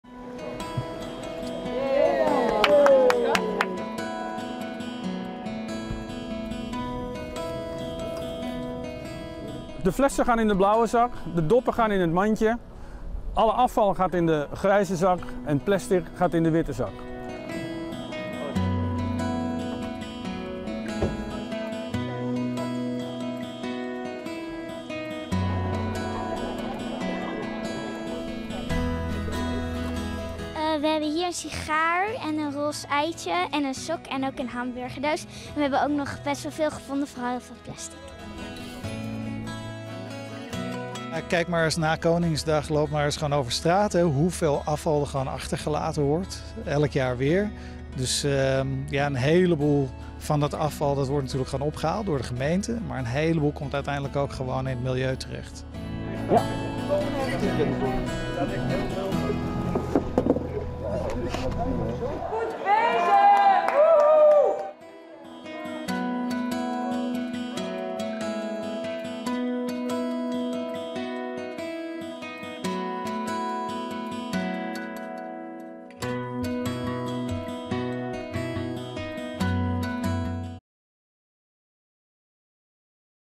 Vrijwilligers vissen Amsterdamse grachten schoon na Koningsdag - RTL NIEUWS
Bijna 300 vrijwilligers hielpen vandaag mee om de grachten van Amsterdam schoon te vissen. Elk jaar belandt er veel troep in de grachten tijdens Koningsdag. De 10-jarige Isa vond een sok, een roze ei een sigaar en nog veel meer.